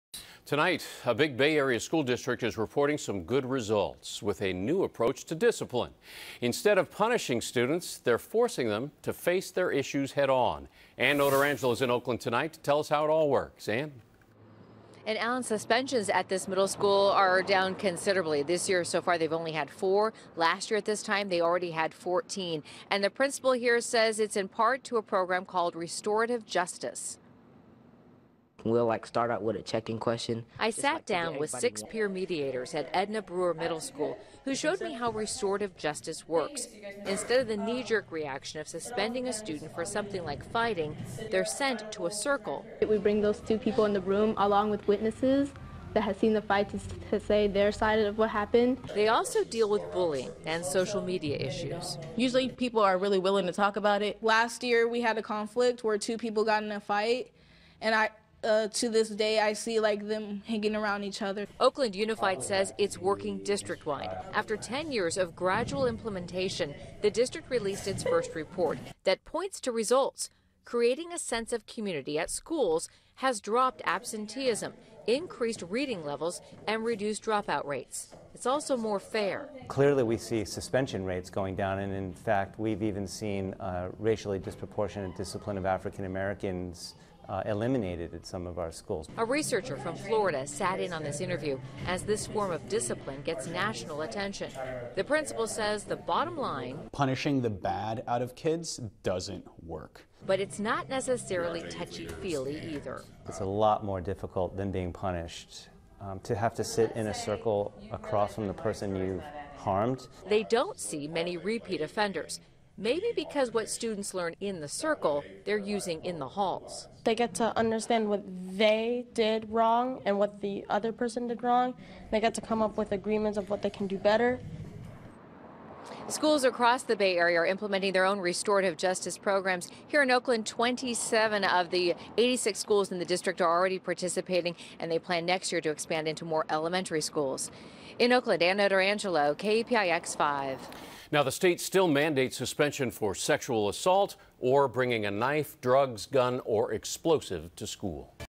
Oakland Schools Find Success With New Approach To Discipline
Oakland Unified School District is reporting good results with a new approach to discipline. Instead of punishing students, they are forcing them to face their issues, head on. Ann Notarangelo reports. (1/14/15)